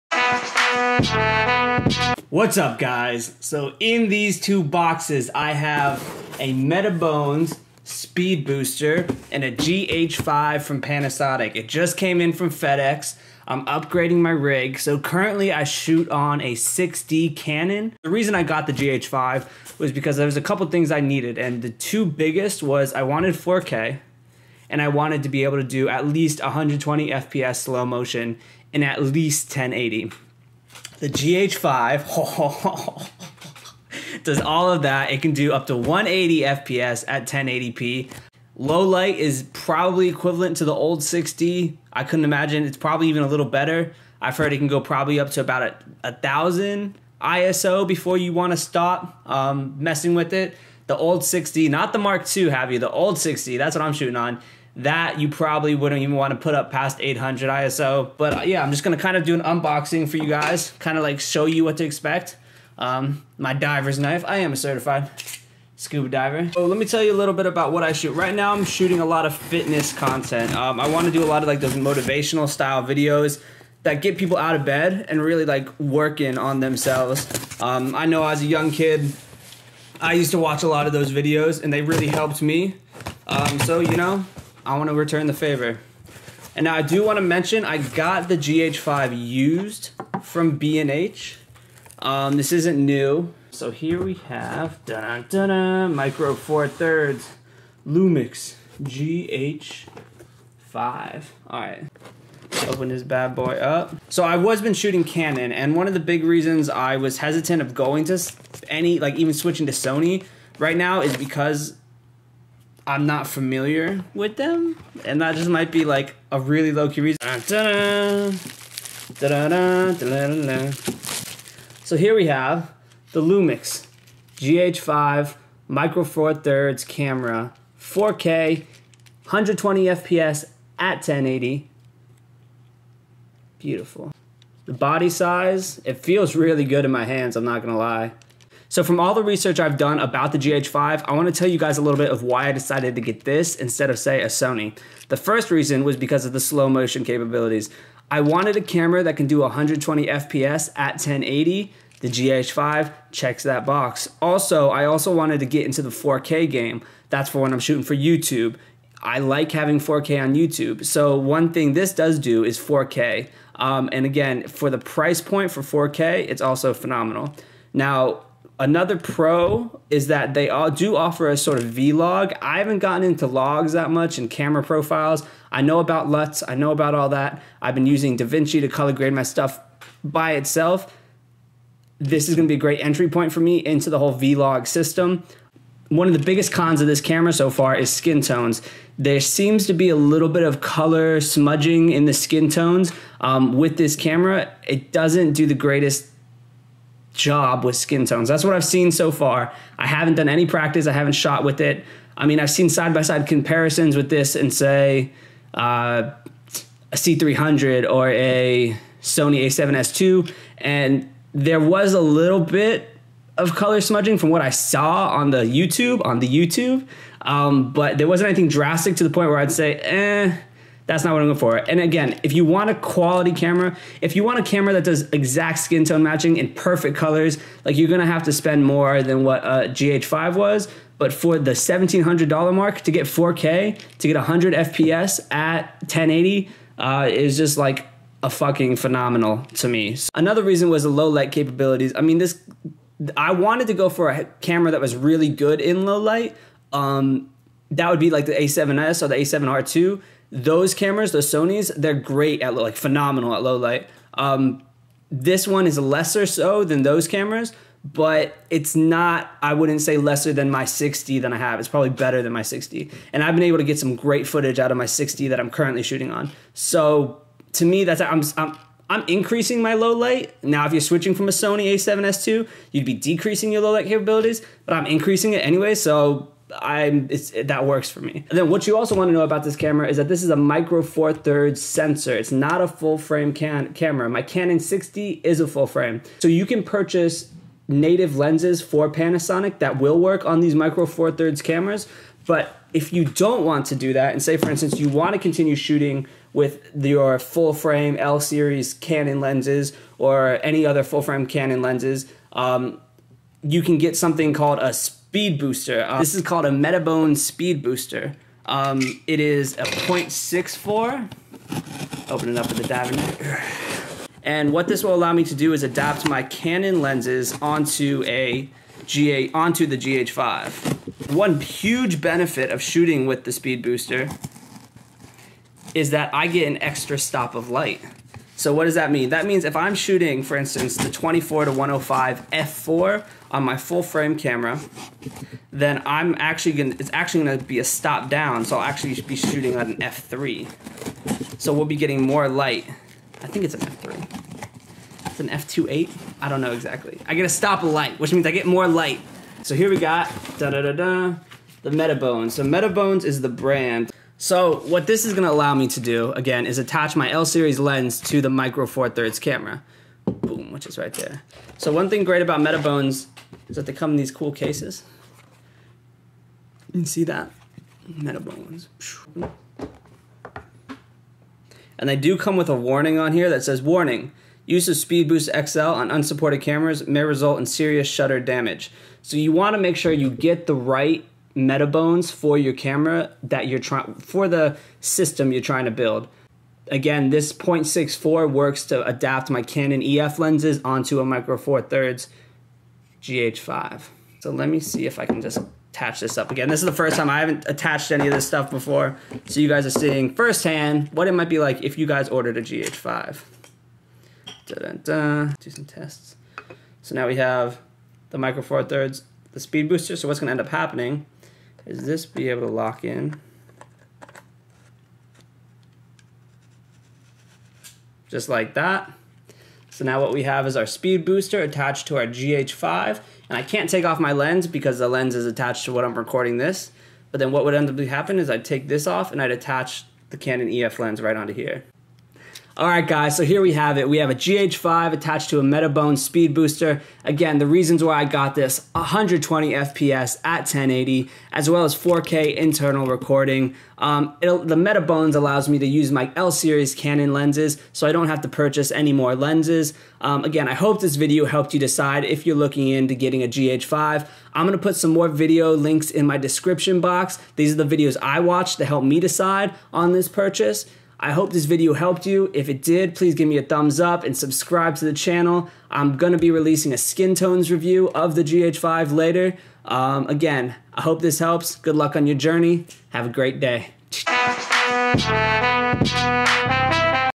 Whats up with the GH5 and METABONES?
I have just purchased a GH5 and a Metabones Speed Booster .64 to adapt my Canon EF lens.

Here is what I learned while researching his purchase as well some unboxing information.

I created this video to help users on their pre-purchasing research.